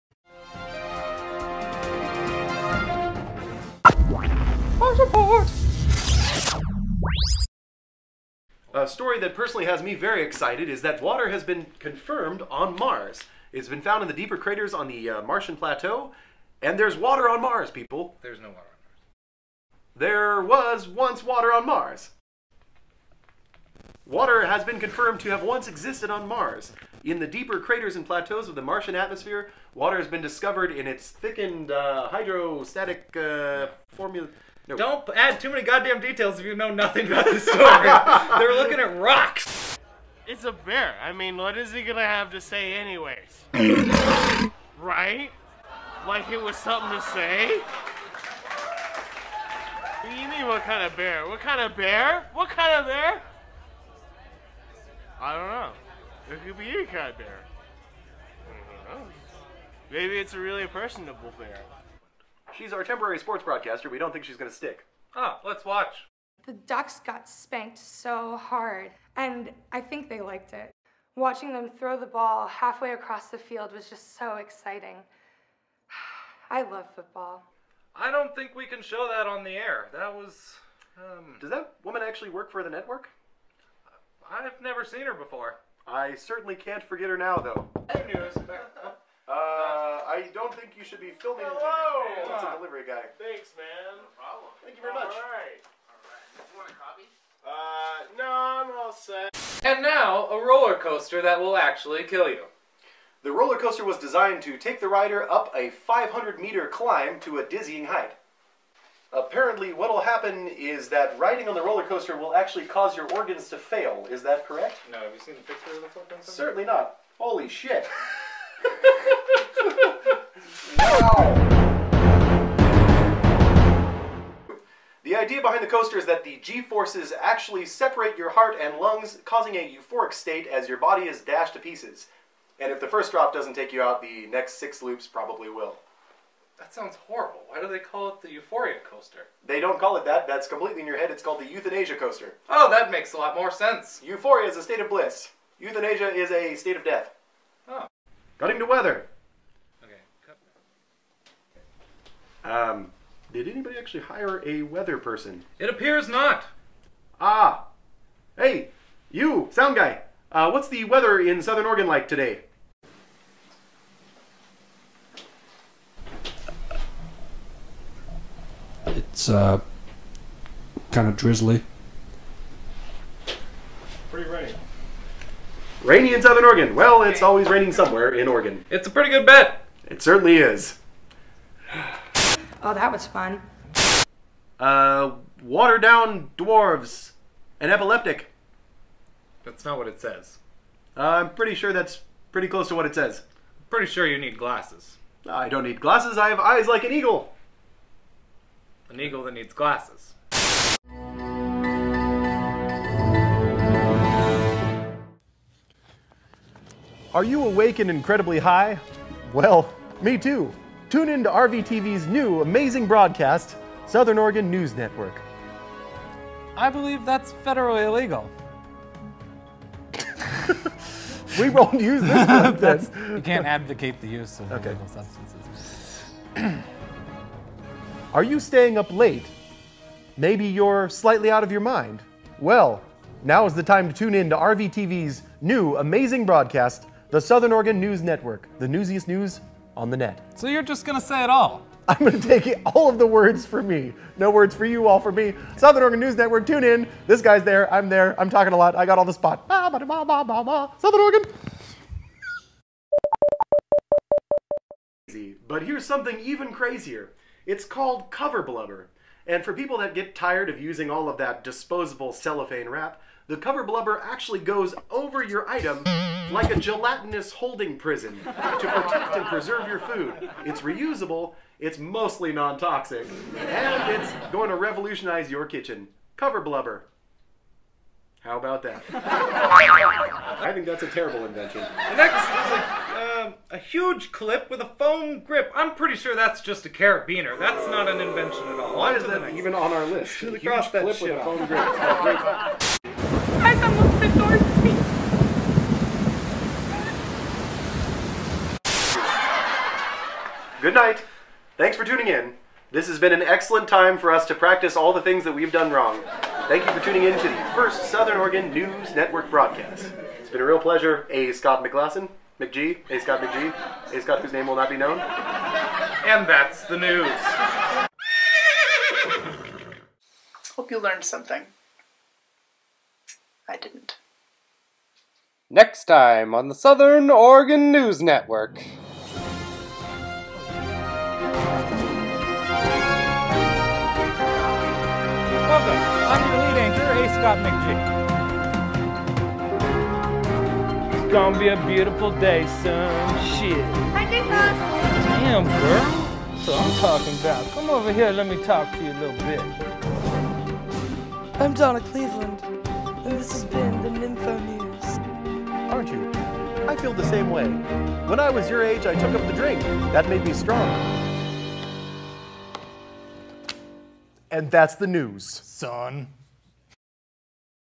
Best Of SONN: Episode 1
All of our favorite clips from the pilot episode filmed in Gordon's living room